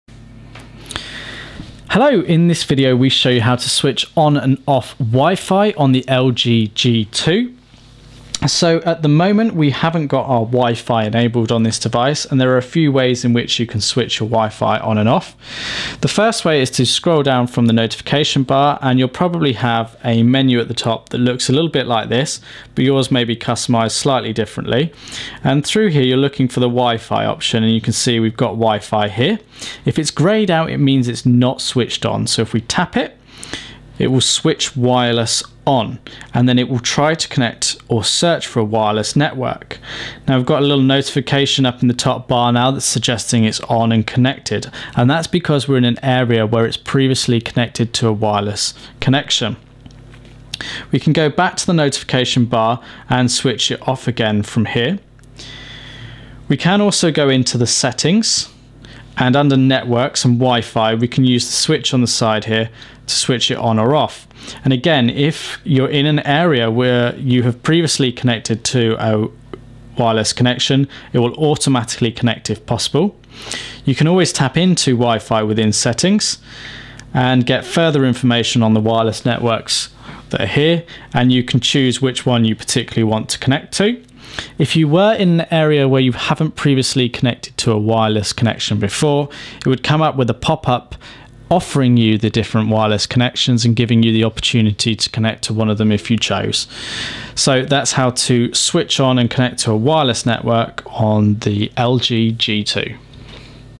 How To Switch WiFi On & Off - LG G2
A video how to, tutorial, guide on switching wifi on and off on the LG G2.